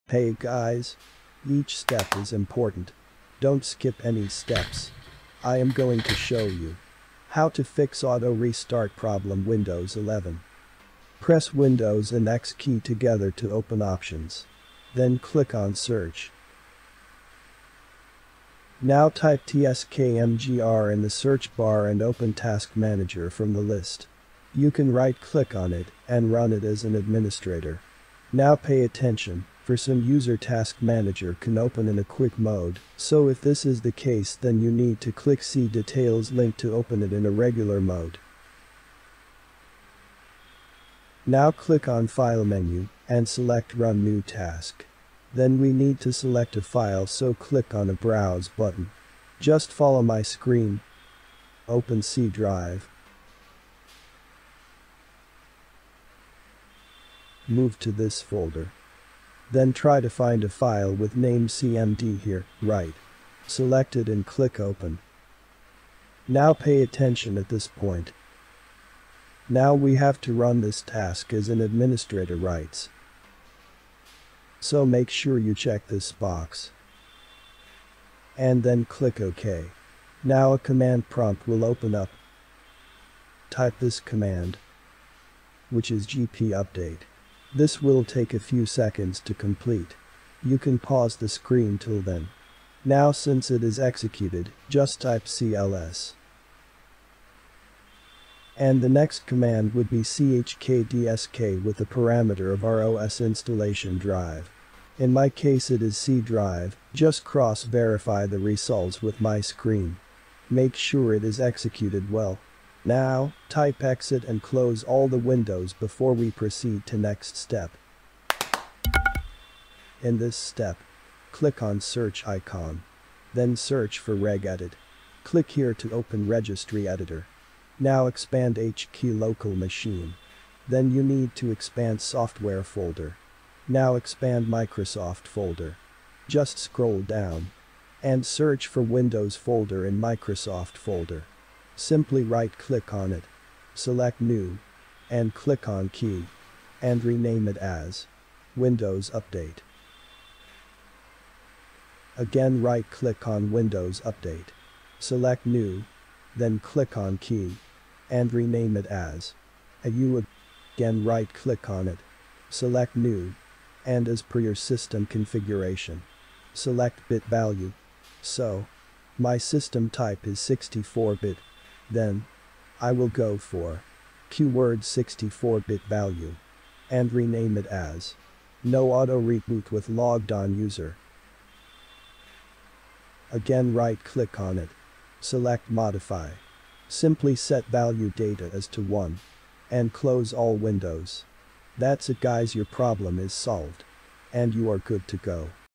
Auto Restart Problem Windows 11 [ How to Fix ] Automatic Startup Problem Windows 11
How to Fix Auto Restart Problem Windows 11
In this video we will see how you can fix your Windows 11 automatic startup issues.

Language : English